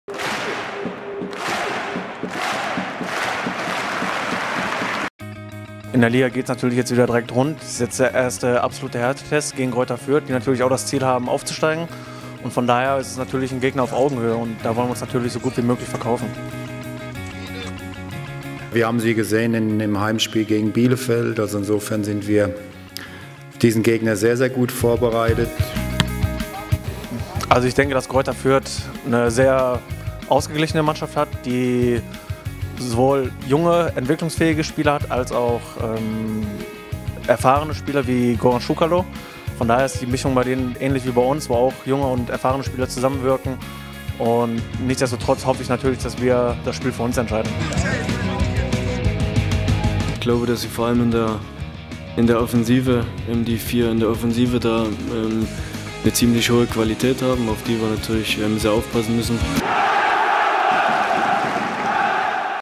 Vorbericht: SpVgg Greuther Fürth - 1. FC Kaiserslautern
Am Montag, 12. August 2013, treten die Roten Teufel beim Tabellenführer in Fürth an. FCK-TV gibt Euch einen kleinen Einblick in die Vorbereitungen und die Stimmung. Den kompletten Clip seht Ihr wie immer